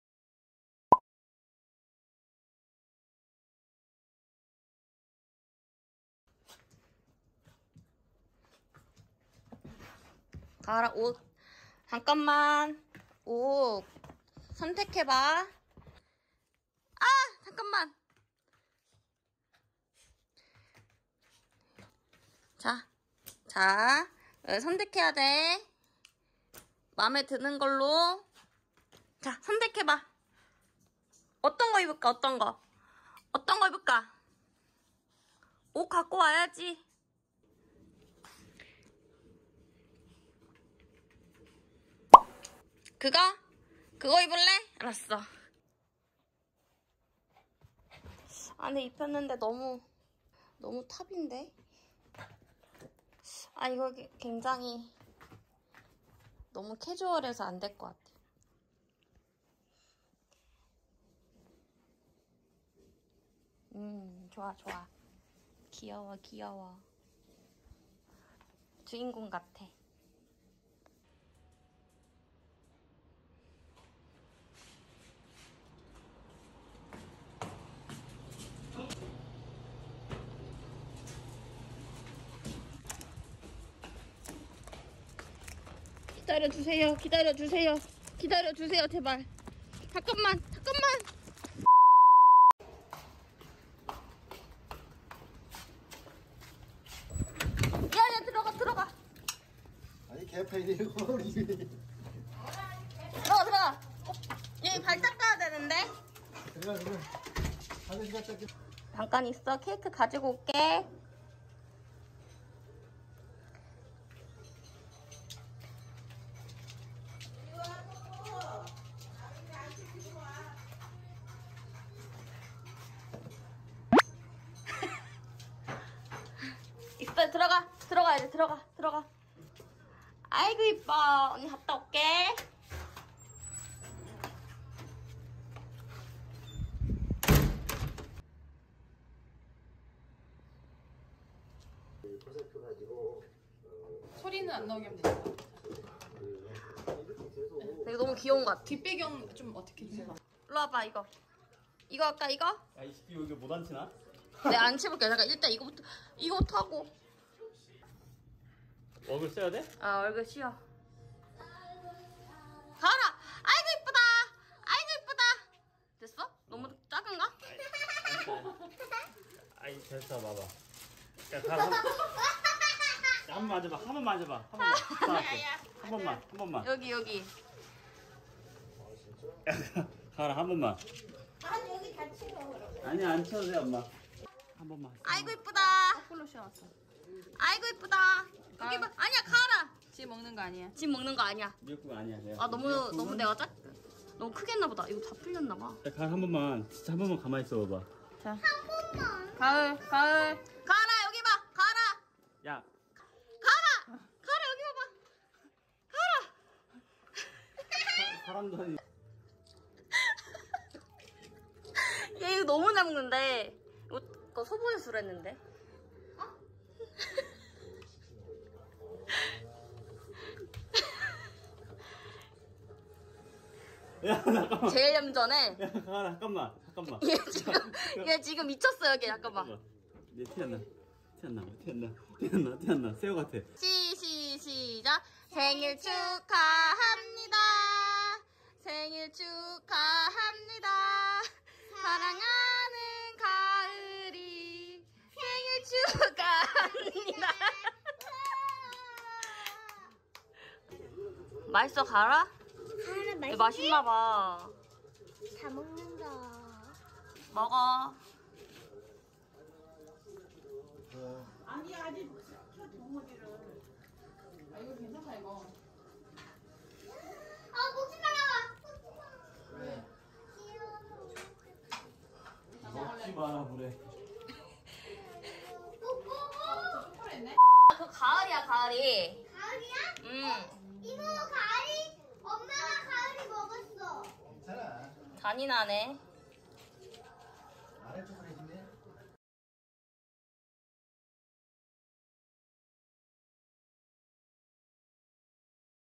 가을이 생일파티를 해요
백신2차 맞고 멈춰버린 편집,,,
전,,,브이로그하기엔 너무 성질이 급하네요...
오늘도 가을이 예뻐해주셔서 감사합니다 S2

이름 : 박가을
성별 : 중성화 완료한 여아